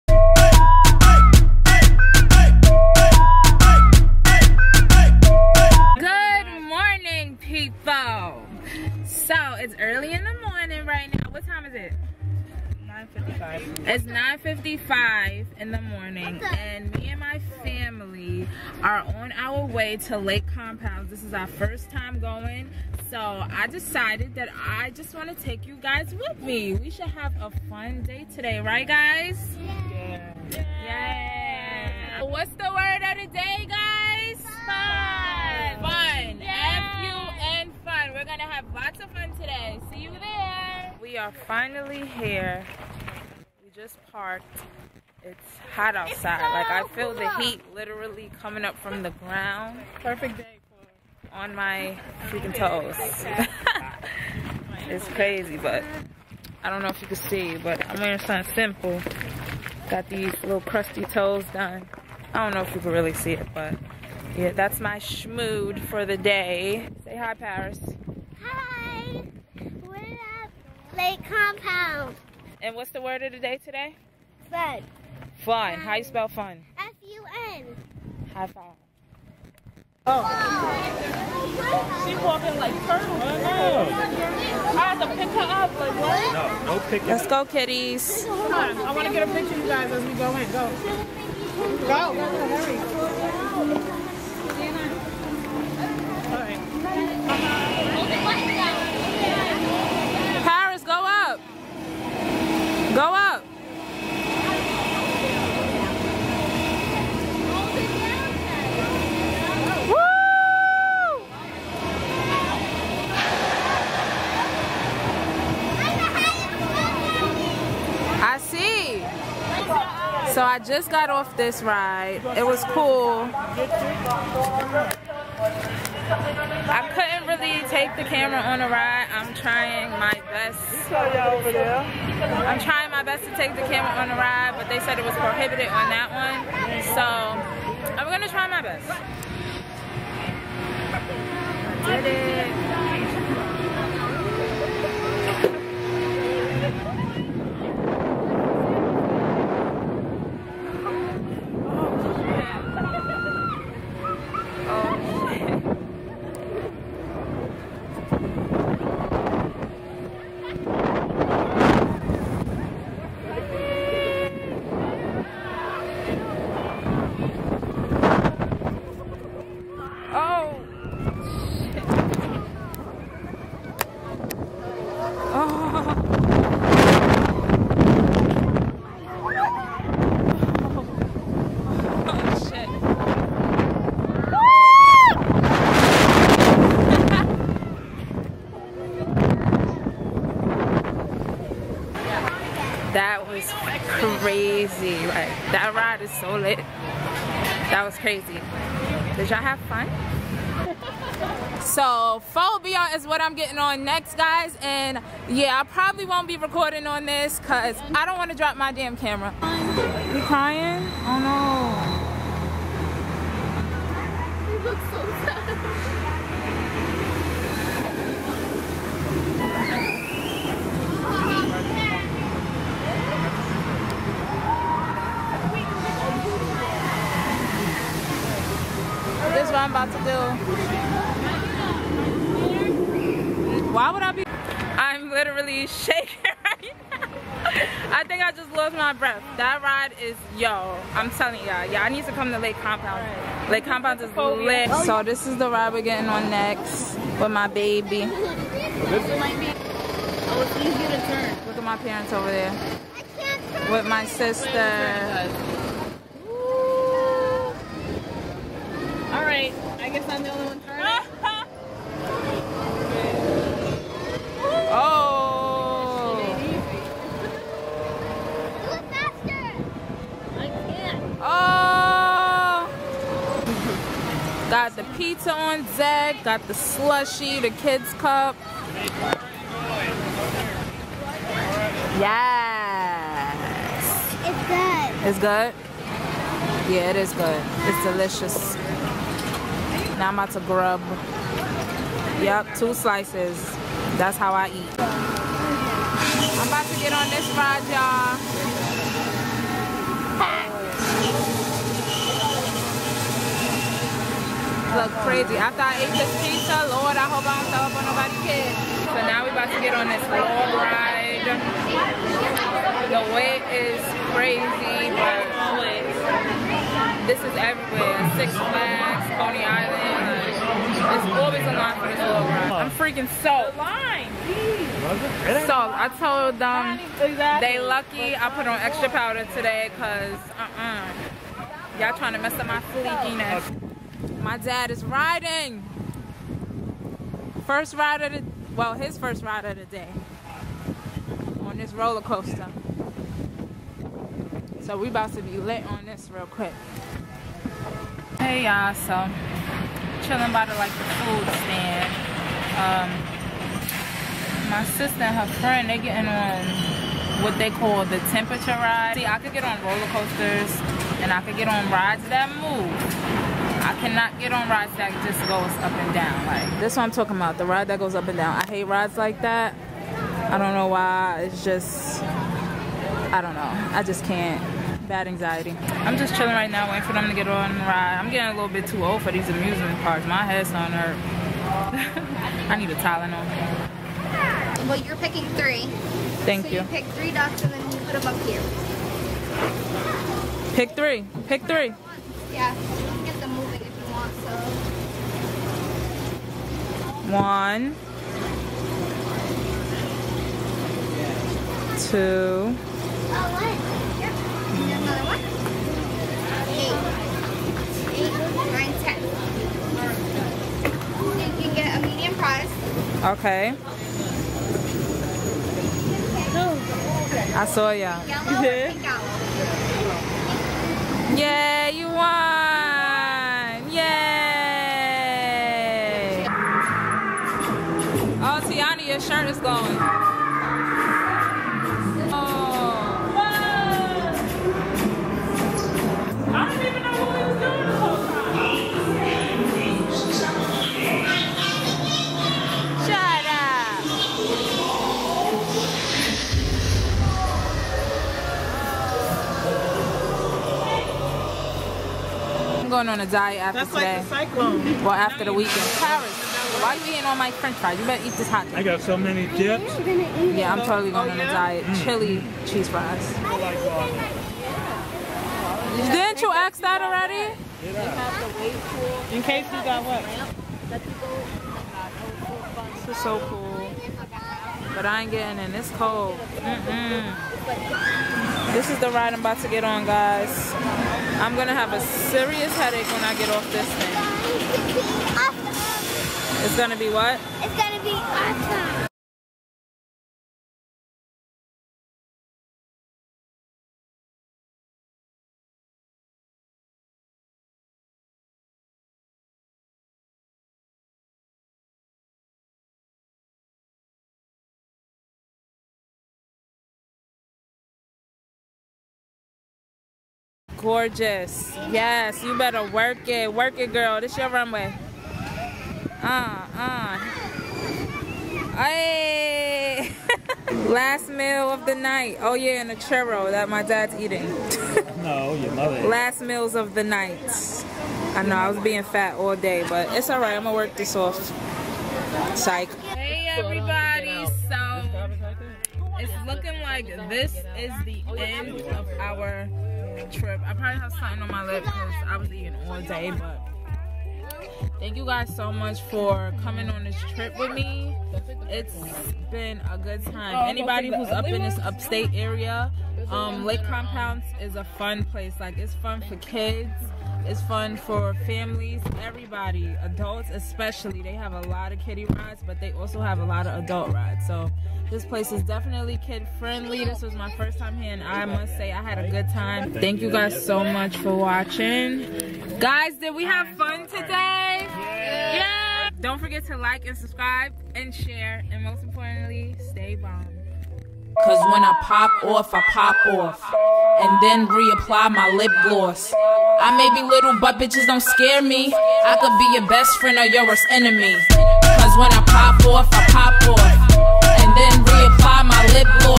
Summer VLOG #3 First Time At Lake Compounce| Bomb Nia
HEY GUYS !

Join me and my family at Lake Compounce for our FIRST TIME !

Get to know the real me . .

Camera Used- Samsung Nx5000 Mirrorless
Program Used-iMovie